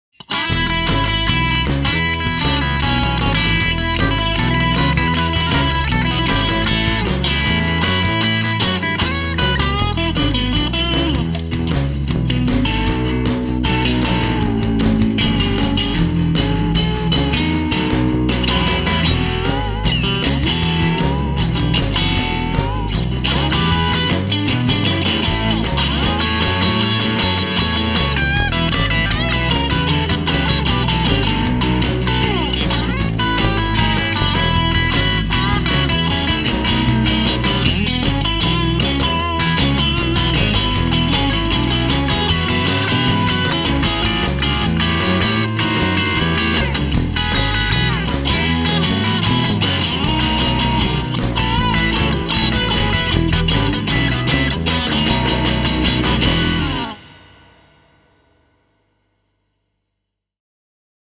Blues inspired by SRV with Kemp Strings Standard 10s on Fender Strat
I love a bit of blues inspired by SRV (plus some tremchords). Tone for days with Standard 10s from kempstrings optimised for strat through kingoftone by analogman